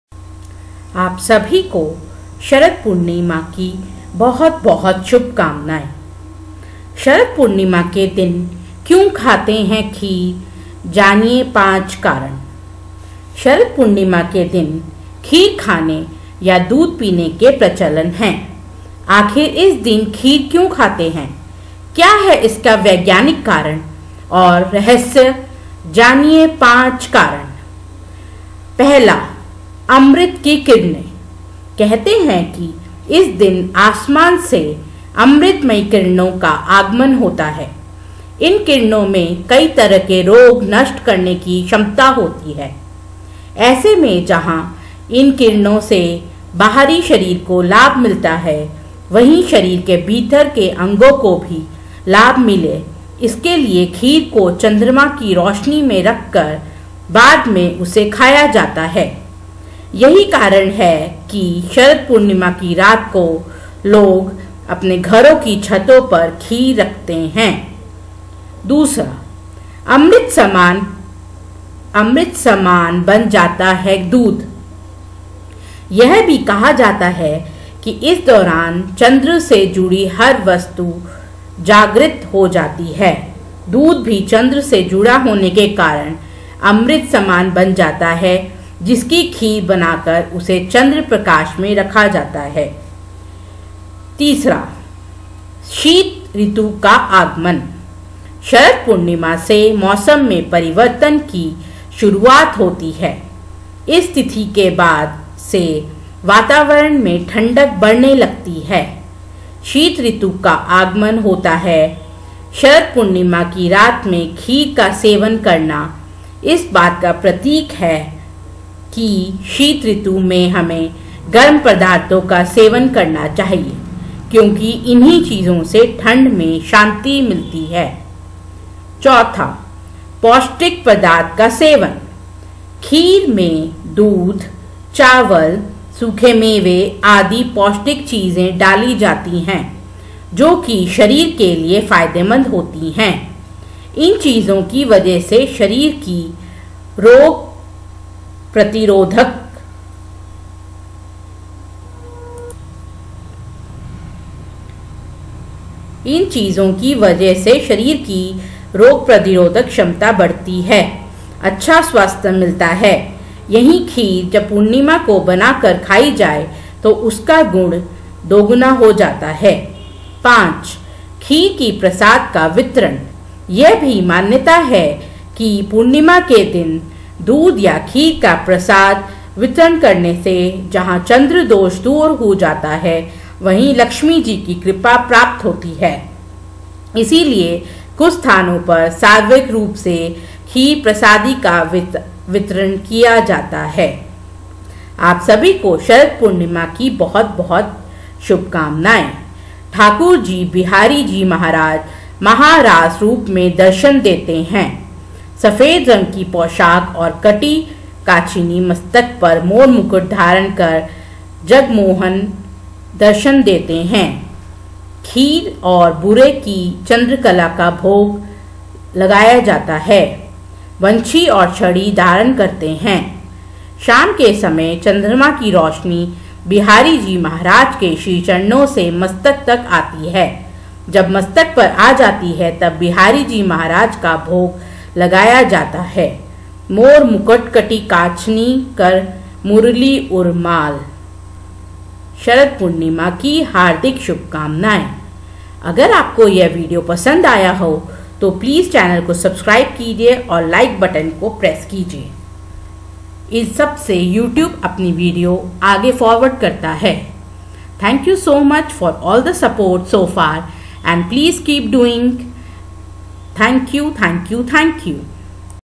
शरद पूर्णिमा के दिन क्यों खाते हैं खीर, जानिए 5 कारण || 5 Things About Sharad Purnima Kheer .
शरद पूर्णिमा के दिन क्यों खाते हैं खीर, जानिए 5 कारण
👉शरद पूर्णिमा के दिन खीर खाने या दूध पीने   के  प्रचलन हैं। आखिर इस दिन खीर क्यों खाते हैं? क्या है इसका वैज्ञानिक कारण और रहस्य जानिए 5 कारण।

1.अमृत की किरणें : कहते हैं कि इस दिन आसमान से अमृतमयी किरणों का आगमन होता है। इन किरणों में कई तरह के रोग नष्ट करने की क्षमता होती है। ऐसे में जहां इन किरणों से बाहरी शरीर को लाभ मिलता है वहीं शरीर के भीतर के अंगों को भी लाभ मिले इसके लिए खीर को चंद्रमा की रोशनी में रखकर बाद में उसे खाया जाता है। यही कारण है कि शरद पूर्णिमा की रात को लोग अपने घरों की छतों पर खीर रखते हैं।

2.अमृत समान बन जाता है दूध : यह भी कहा जाता है कि इस दौरान चंद्र से जुड़ी हर वस्तु जाग्रत हो जाती है। दूध भी चंद्र से जुड़ा होने ने कारण अमृत समान बन जाता है जिसकी खीर बनाकर उसे चंद्रप्रकाश में रखा जाता है।

3.शीत ऋतु का आगमन : शरद पूर्णिमा से मौसम में परिवर्तन की शुरूआत होती है। इस तिथि के बाद से वातावरण में ठंडक बढ़ने लगती है। शीत ऋतु का आगमन होता है। शरद पूर्णिमा की रात में खीर का सेवन करना इस बात का प्रतीक है कि शीत ऋतु में हमें गर्म पदार्थों का सेवन करना चाहिए, क्योंकि इन्हीं चीजों से ठंड में शक्ति मिलती है।

4.पौष्टिक पदार्थ का सेवन : खीर में दूध, चावल, सूखे मेवे आदि पौष्टिक चीजें डाली डाती हैं, जो कि शरीर के लिए फायदेमंद होती हैं। इन चीजों की वजह से शरीर की रोगप्रतिरोधक क्षमता बढ़ती है। अच्छा स्वास्थ्य मिलता है। यहीं खीर जब पूर्णिमा को बनाकर खाई जाए तो उसका गुण दोगुना हो जाता है।

5.खीर की प्रसाद का वितरण : यह भी मान्यता है कि पूर्णिमा के दिन दूध या खीर का प्रसाद वितरण करने से जहां चंद्रदोष दूर हो जाता है वहीं लक्ष्मी की कृपा प्राप्त होती है। इसीलिए कुछ स्थानों पर सार्वजनिक रूप से खीर प्रसादी का वितरण किया जाता है❤❤❤❤❤❤❤❤❤❤